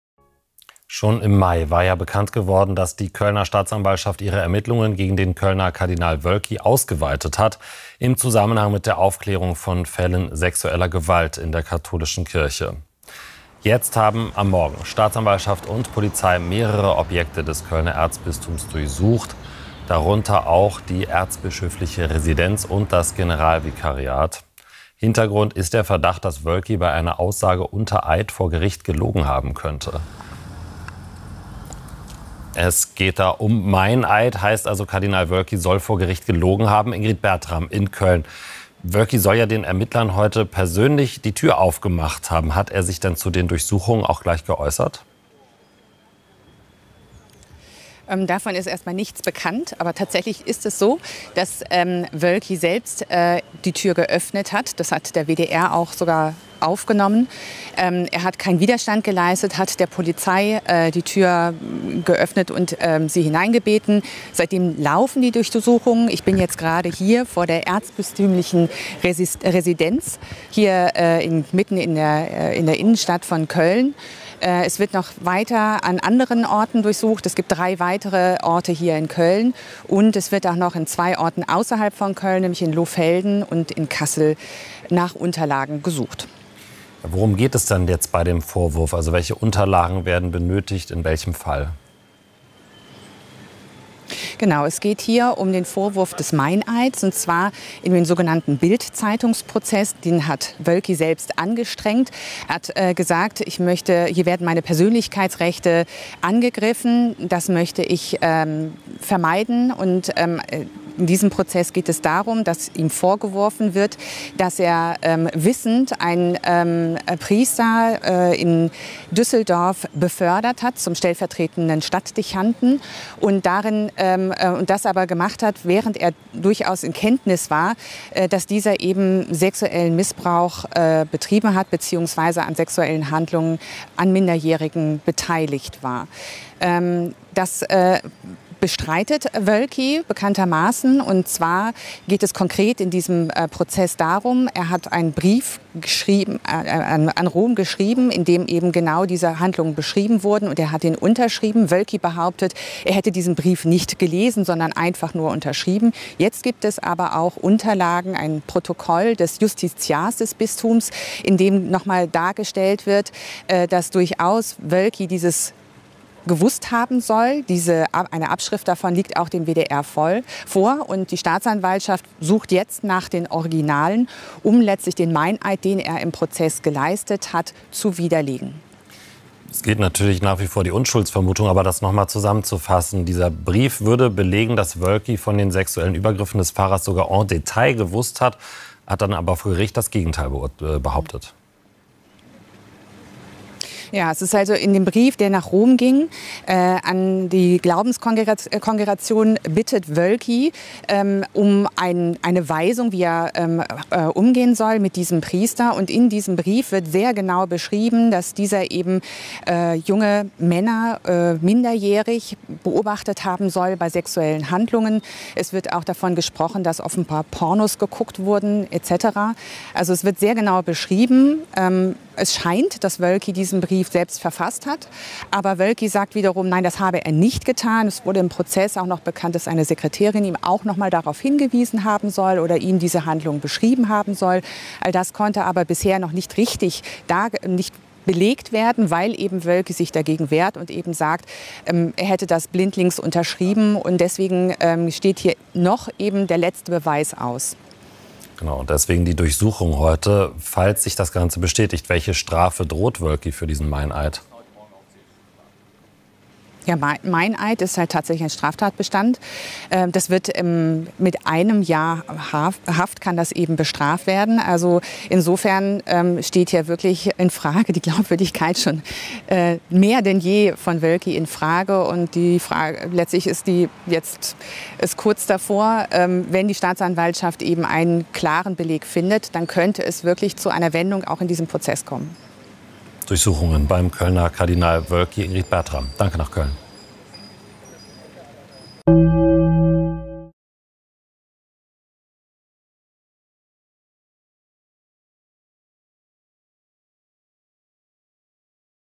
Ermittlungen gegen Woelki: Razzia im Erzbistum Köln
Seit den Morgenstunden durchsuchen Kölner Staatsanwaltschaft und Polizei verschiedene Objekte im Erzbistum Köln. Hintergrund der Razzia sind Meineid-Ermittlungen gegen den Kölner Erzbischof Rainer Maria Woelki. Die Staatsanwaltschaft sucht die Originale von belastenden Dokumenten, die der WDR in den vergangenen Monaten bereits veröffentlicht hat. Sie legen den Verdacht nahe, dass Woelki vor Gericht gelogen hat.